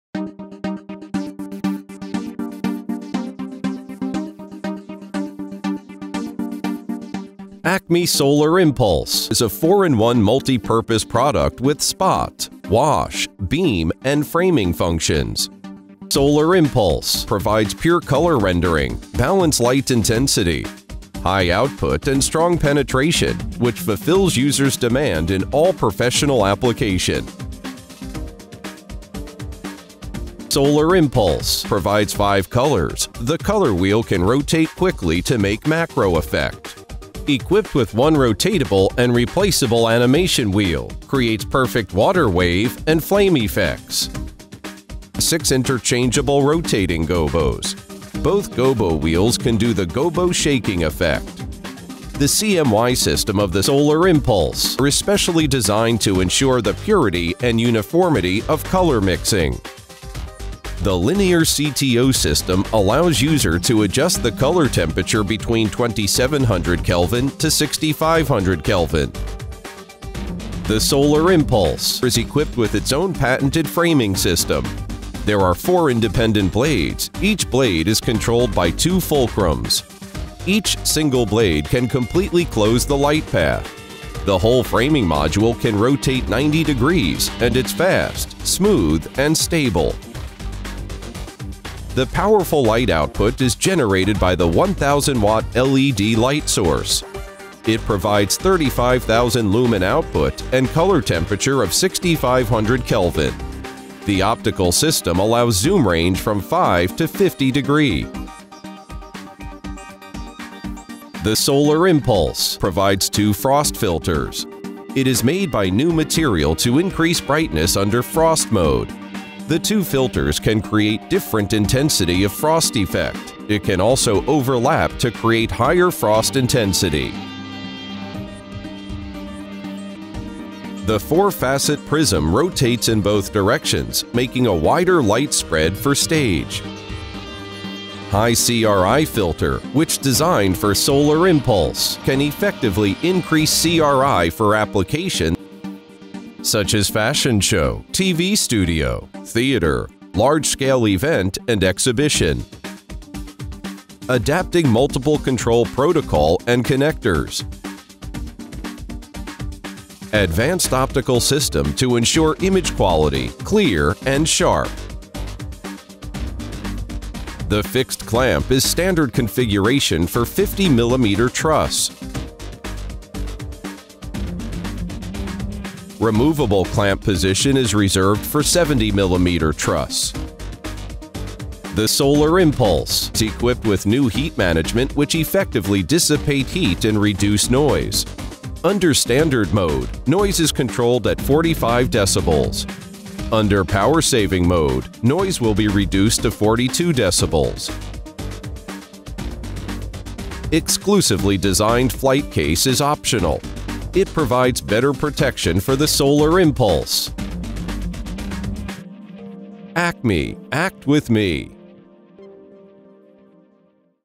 ACME SOLAR IMPULSE
ACME’s milestone product, the SOLAR IMPULSE, with its break-through 1000W white LED technology,produces an output of over 35,000lumens, and is the perfect replacement for all max-strength spotlight fixtures with discharge lamps up to 1400W.

The SOLAR IMPULSE masterfully combines Beam,Spot andWash capability with a 4-blade framing shutter - making four functions in one solidly-build unit. The 5-50˚(1:10 ratio) zoom is smooth and consistent throughout the entire range, with no hitches or pauses. With its CRI 90/70 switch and choice of light or heavy frost filters, it is an attractive option for various applications including video shoots, fashion showsand more.